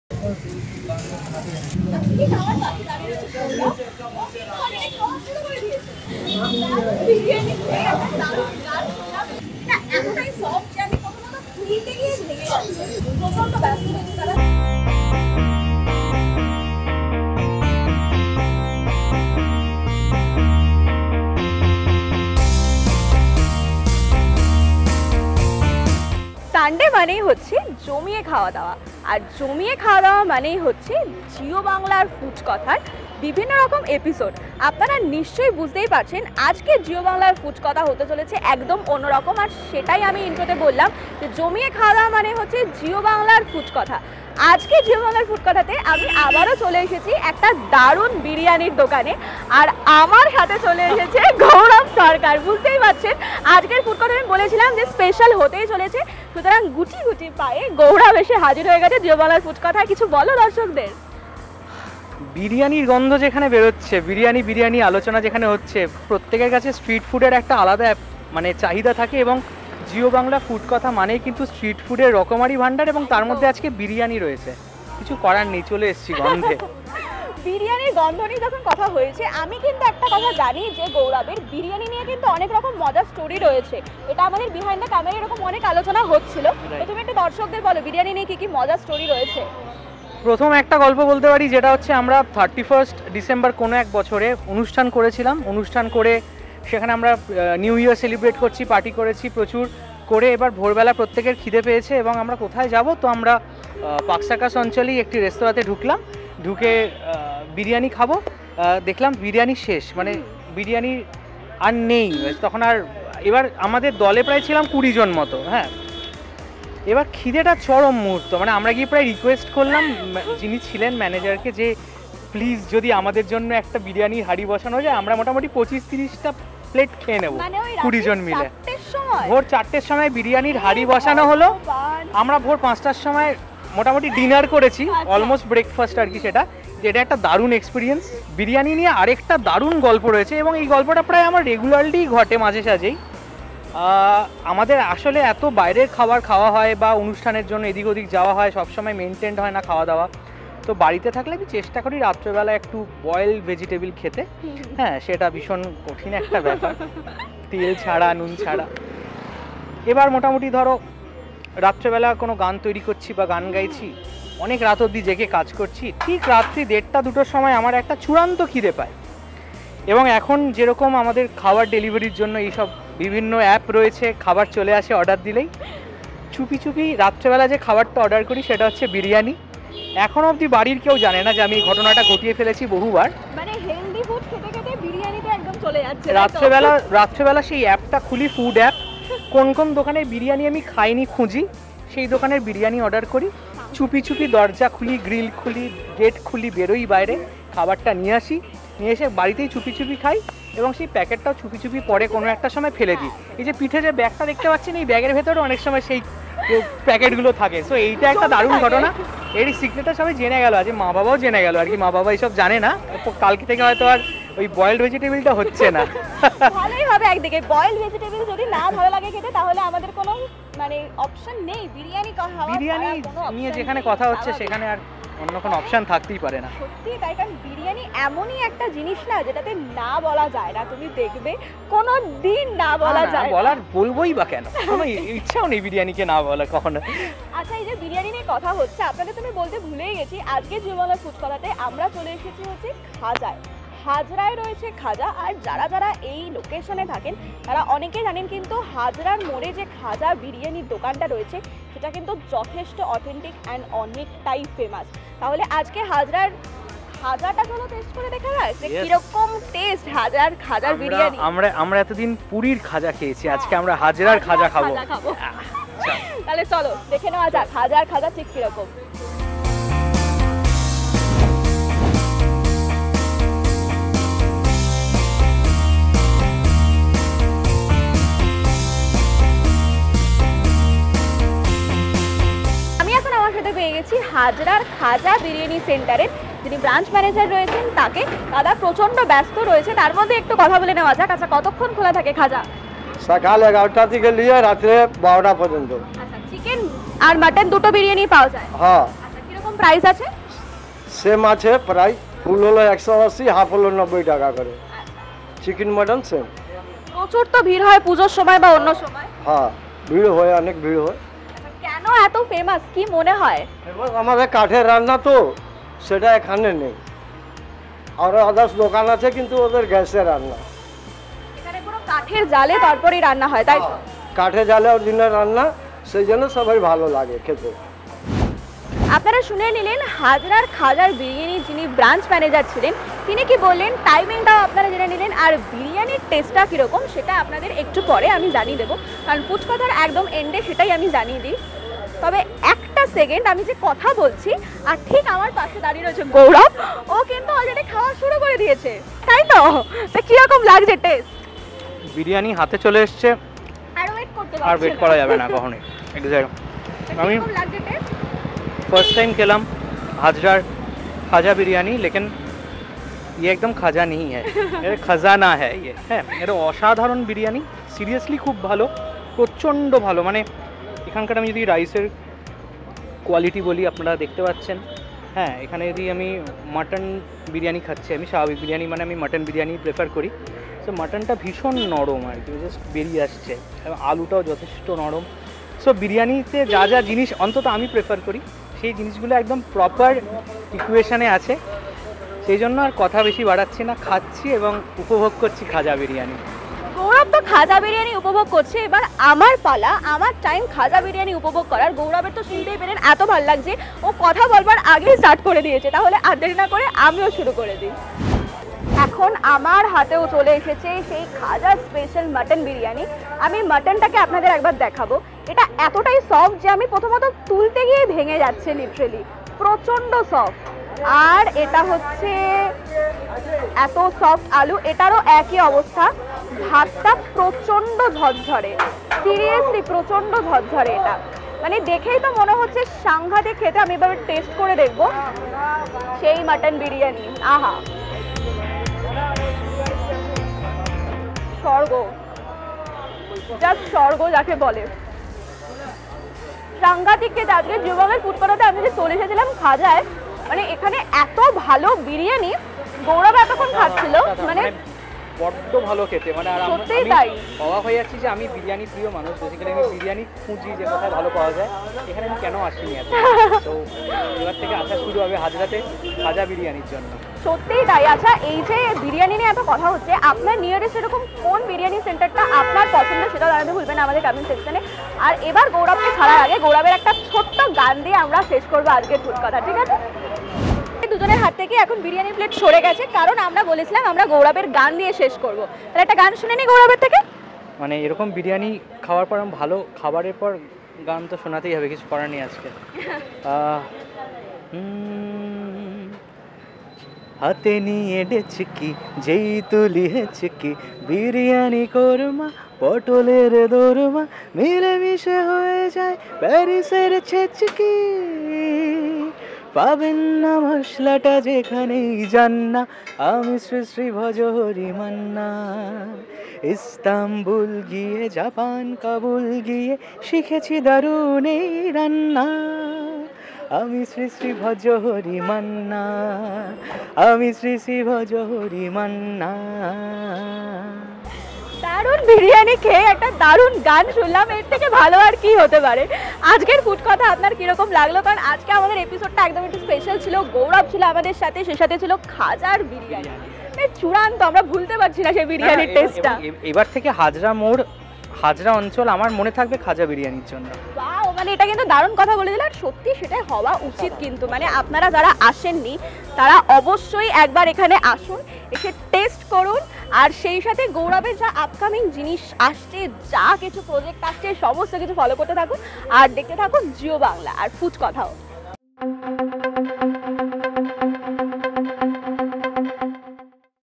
Khaja Biryani Hazra | মাত্র 90 টাকা Mutton Biryani | Singer Gourab Sarkar | Food Katha | Jiyo Bangla
দারুণ স্বাদে বিরিয়ানির টানে জিয়ো বাংলার ফুড কথায় হাজির গৌরব সরকার!!! খাজার বিরিয়ানি খেয়ে এ কেমন রিয়াকশন তাঁর!! জানতে হলে ক্লিক করুন নিচের ভিডিওতে...!!

Singer @GourabSarkar on Jiyo Bangla Food Katha!!

Khaja Biryani Centre at Hazra, Kalighat, Kolkata!!